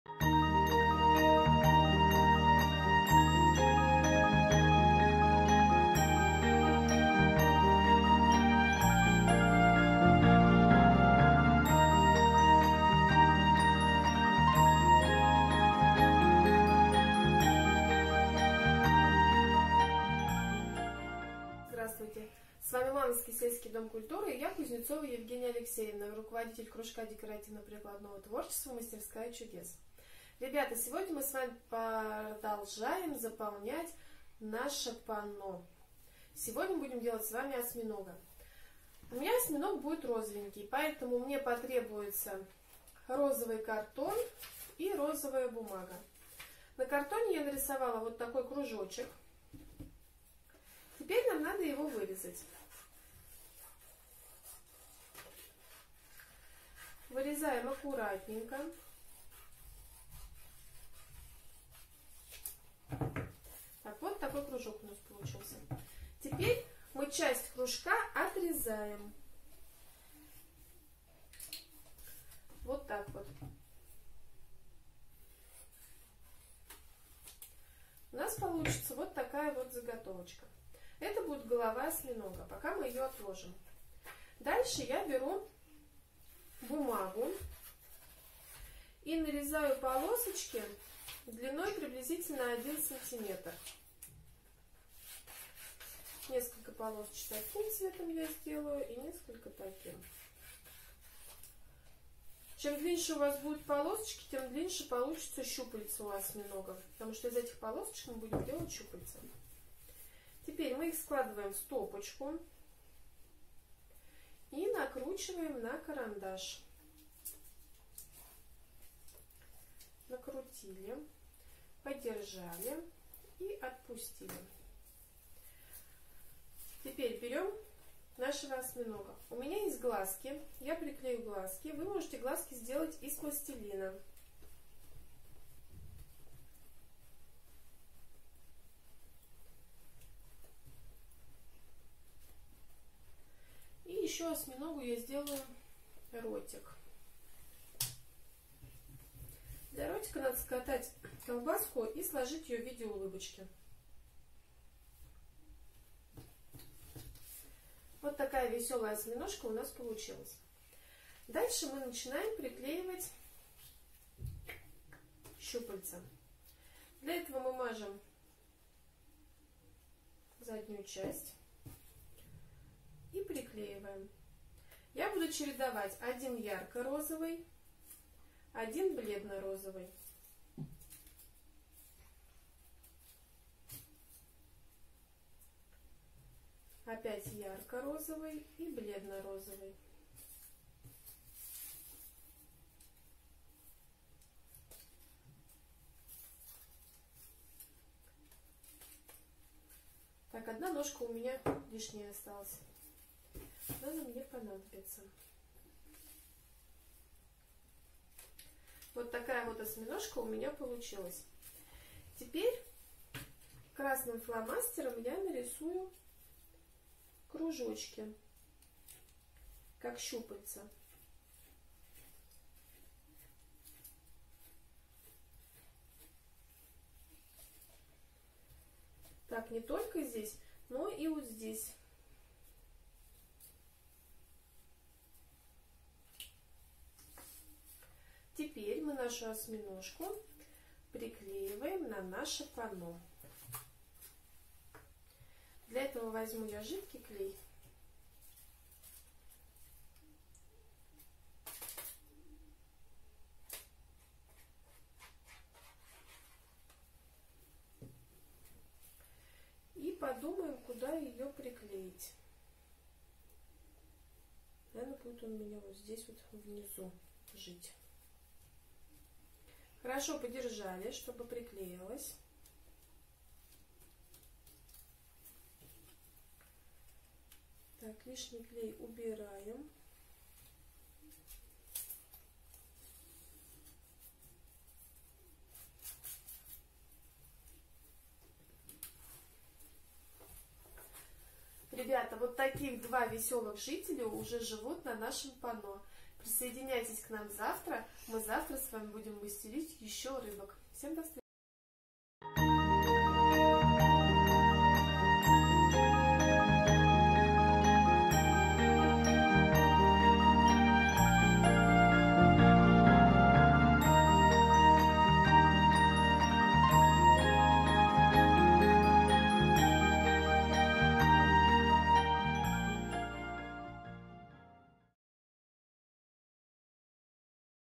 Мастерская чудес мастер – класс «Подводный мир» Часть 3
Мастер – класс от кружка декоративно – прикладного творчества «Мастерская чудес». Руководитель Кузнецова Евгения Алексеевна. На этом мастер-классе мы с вами смастерим панно «Подводный мир». Присоединяйтесь к нам! Будет весело!